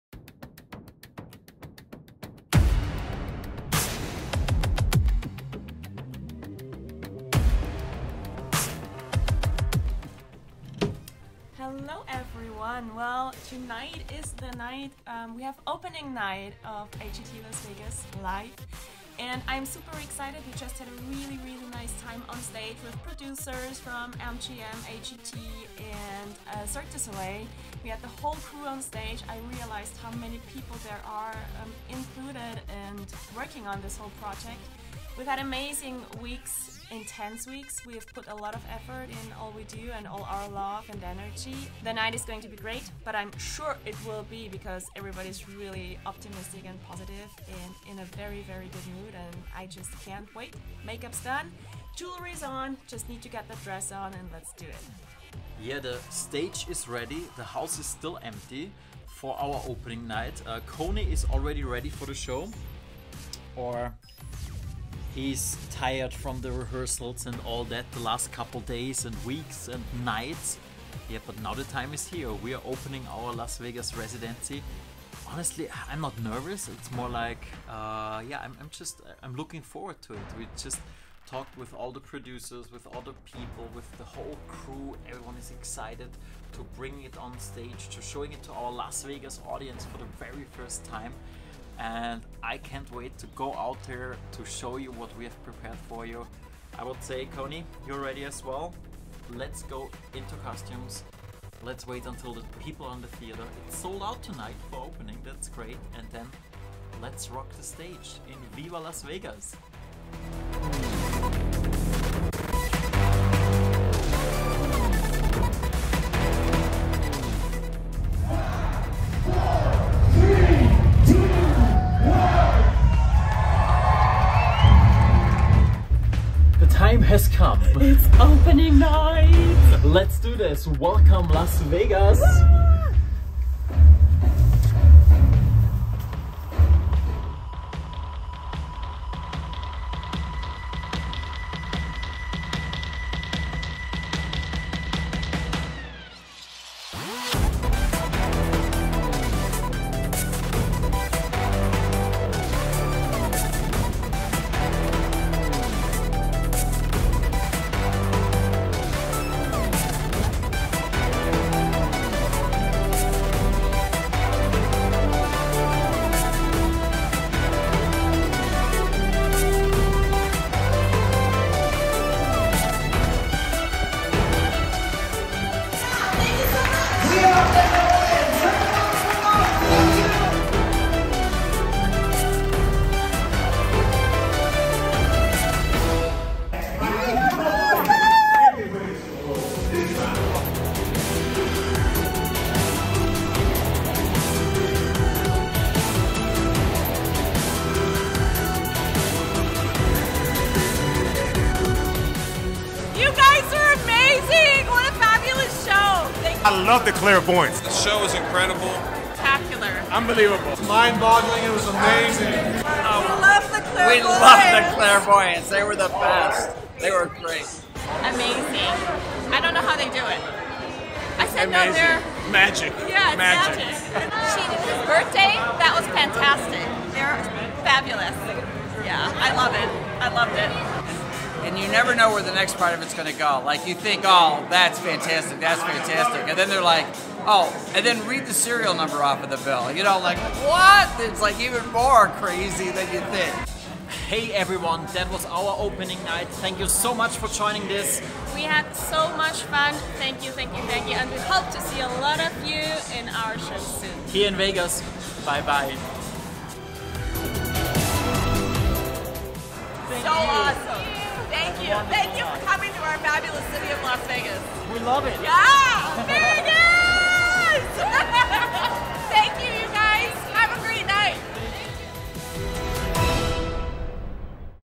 Americas Got Talent LIVE Las Vegas - Opening Night - The Clairvoyants
The Clairvoyants are thrilled to announce that they will be a part of the cast for America’s Got Talent Las Vegas Live residency. The show will opened November 4 2021 at the Luxor Hotel & Casino, where the duo will be performing 2 shows a night, 5 nights a week.

“Performing in Vegas as a headlining act was our dream when we auditioned for AGT in 2016 and now this dream becomes a reality!” Thommy Ten said. “Having the show at the beautiful Luxor Hotel is such an honor and we can’t wait to see all our fans in Las Vegas!”

“We are super trilled performing back live on AGT this August and headlining the AGT Vegas show for 1 year!” Amélie van Tass said. “This is the biggest step in our career and we are ready to welcome our Vegas audience!”

Shows nightly Wednesday - Sunday 7 & 9:30 PM